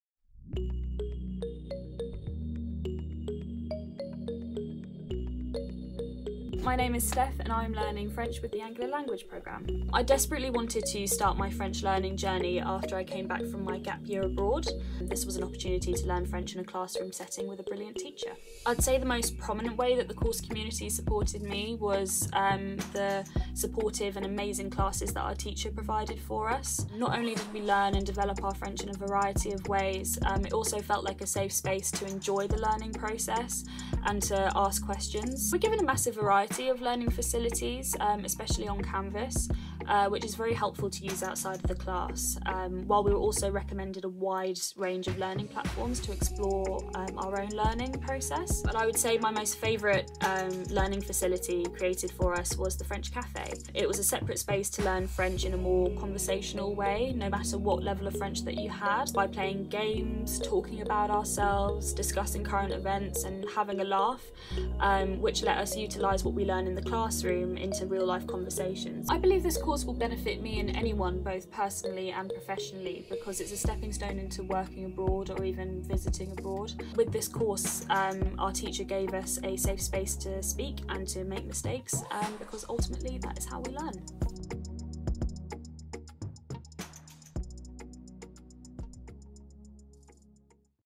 Steph - learning French with a brilliant teacher
ARU BA (Hons) English Literature student Steph talks about why she chose to take French with the Anglia Language Programme, including the supportive community, massive variety of learning materials and the brilliant teacher.